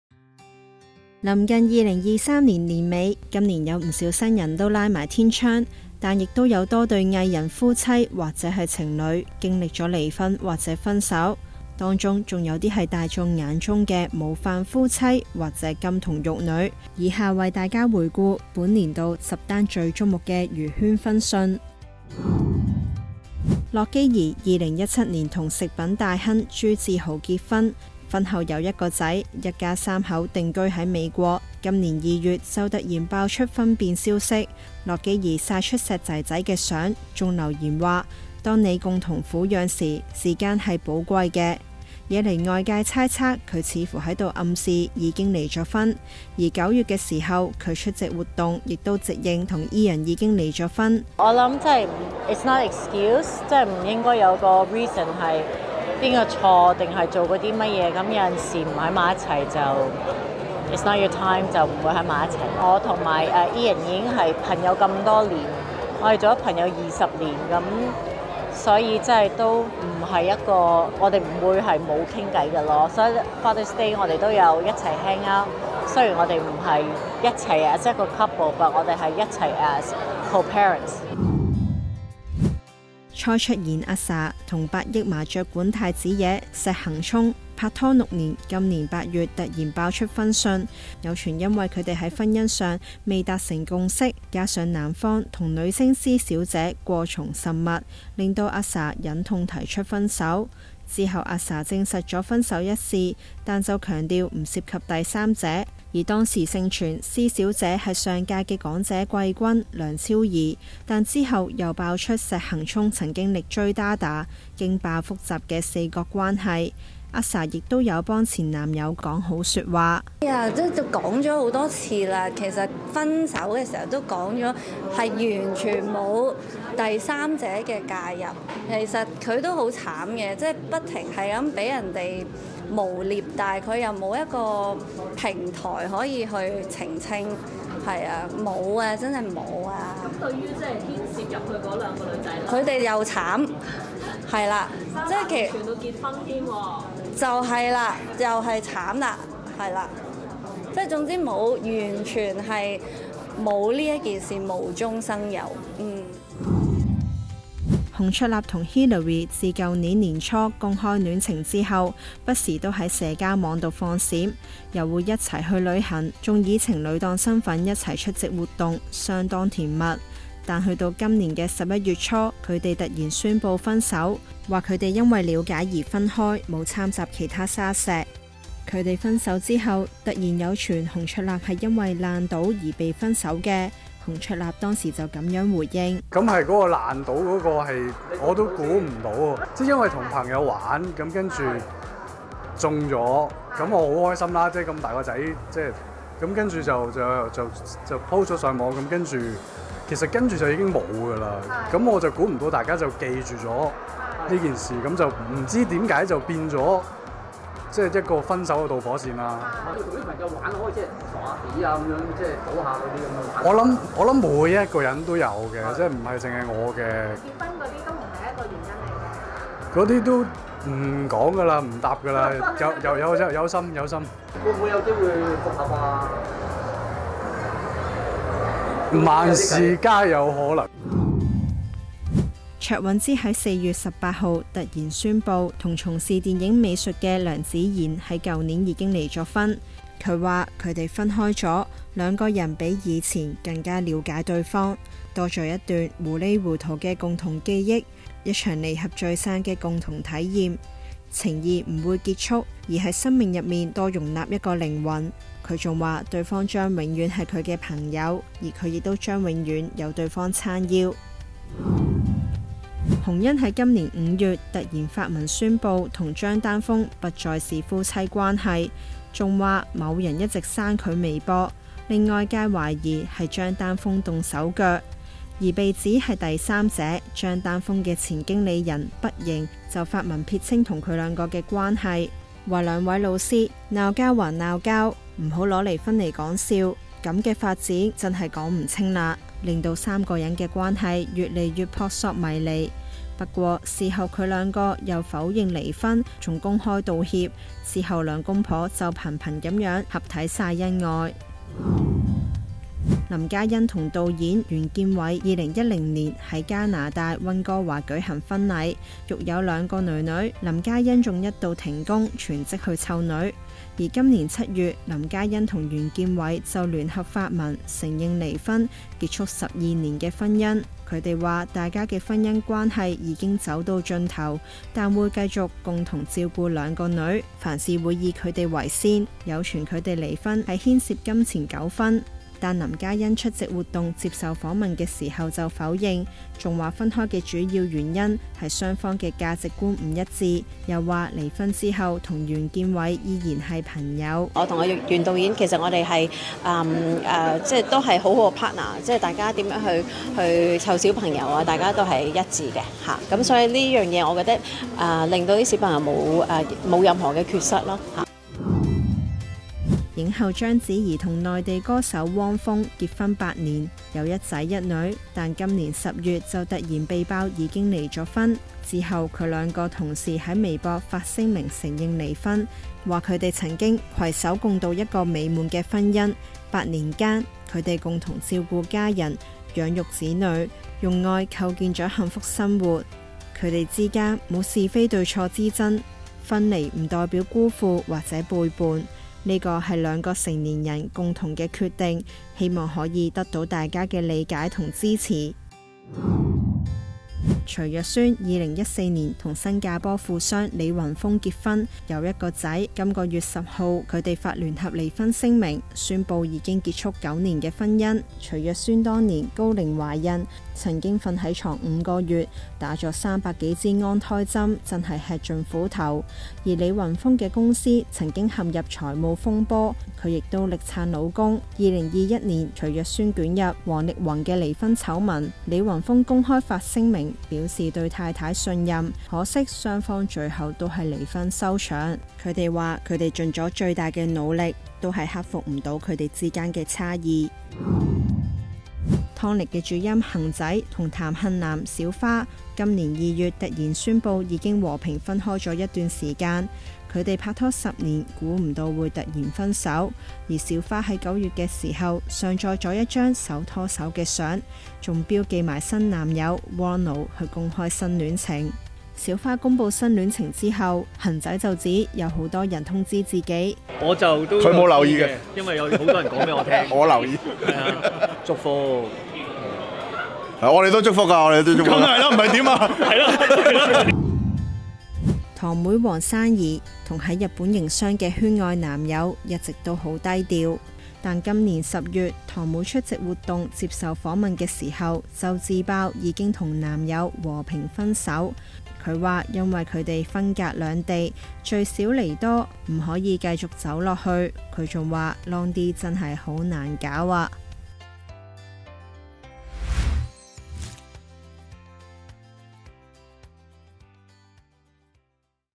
【2023娛樂大事回顧】10對藝人夫妻或情侶經歷離婚或分手 蔡卓妍傳被撬牆腳有人宣布離婚後突然反口｜01娛樂｜蔡卓妍｜洪卓立｜劉沛蘅
【2023娛樂大事回顧】10對藝人夫妻或情侶經歷離婚或分手　蔡卓妍傳被撬牆腳有人宣布離婚後突然反口
臨近2023年年尾，今年有不少新人拉埋天窗，但亦有多對藝人夫妻或情侶經歷離婚或分手，當中更有大眾眼中的模範夫妻或金童玉女，令外界感慨萬分，以下為大家回顧本年10單最矚目的娛圈「分」訊。